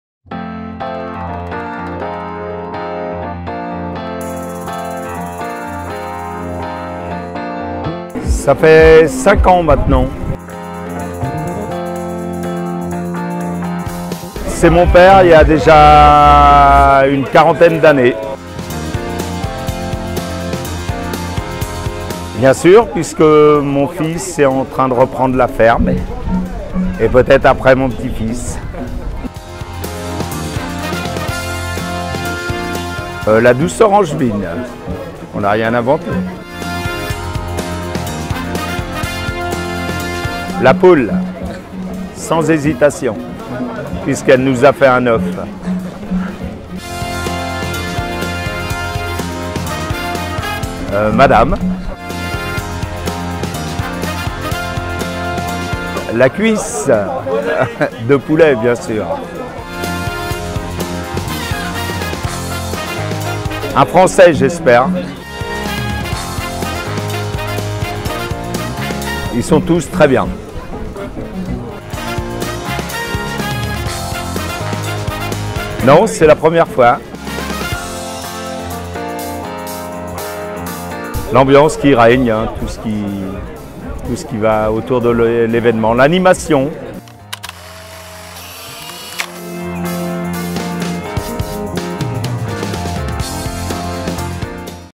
Rencontre avec Daniel, éleveur de poulets Le Gaulois dans le Maine-et-Loire - Tour de France 2018
Faites la connaissance de Daniel, éleveur de poulets Le Gaulois dans le Maine-et-Loire ! . Pour lui, l'élevage, c'est une affaire de famille !

Le Gaulois fait le Tour de France ! 🐓🚴